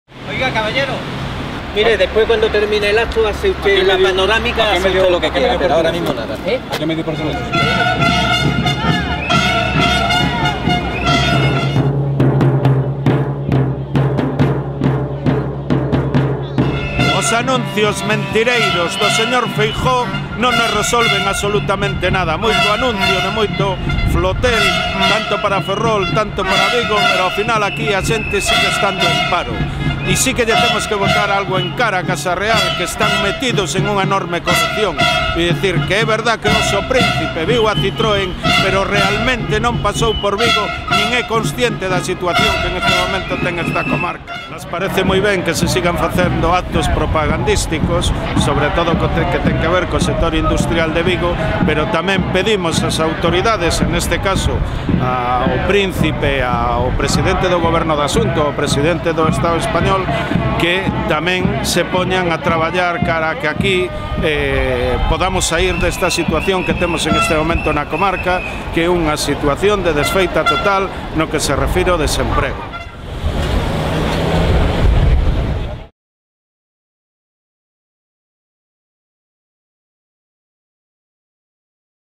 Protestas durante a visita do príncipe español a PSA-Citroën en Vigo (26/9/13)
Militantes da CIG, preferentistas e diversos activistas protestaron este 26 de setembro durante a visita do Príncipe español á factoría de PSA-Citroën na cidade olívica.